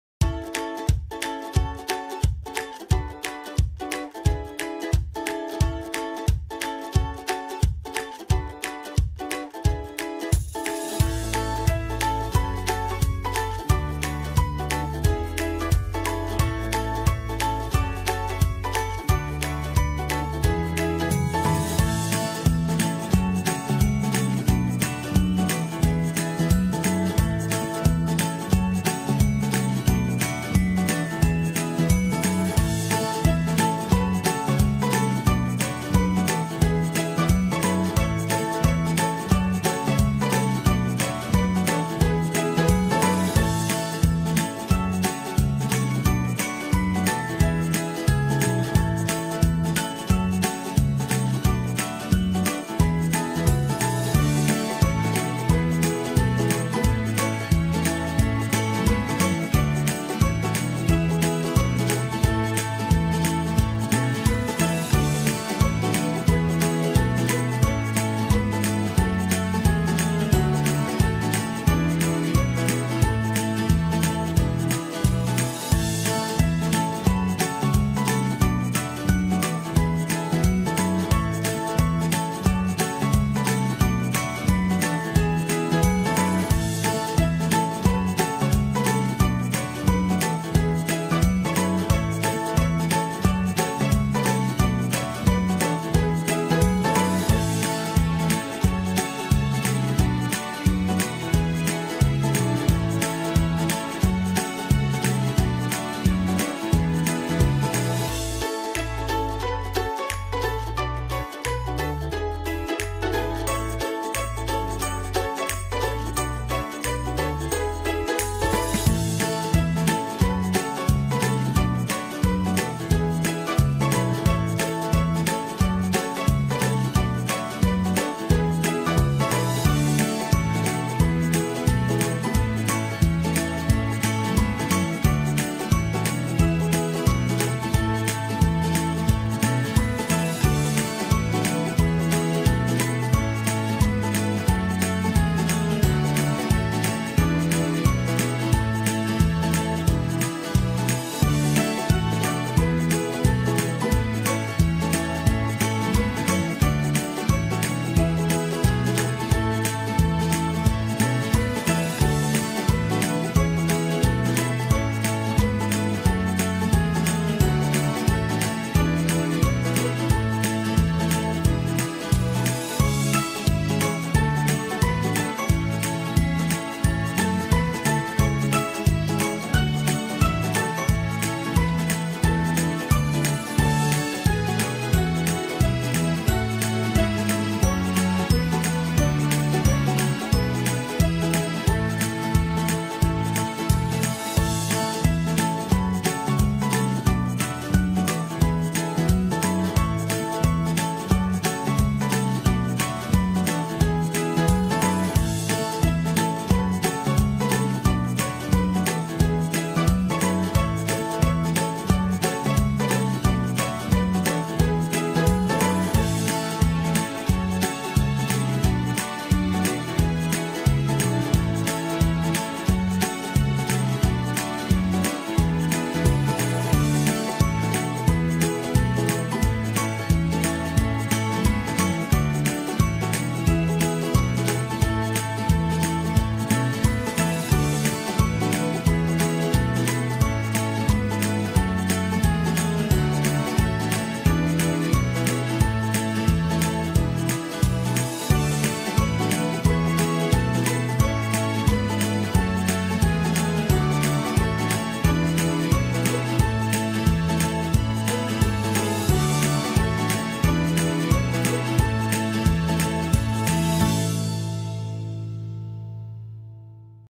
Moisson de blé avec une toute nouvelle John Deere T660i!!!!
Moisson de blé en 2016 avec une toute nouvelle John Deere T660i...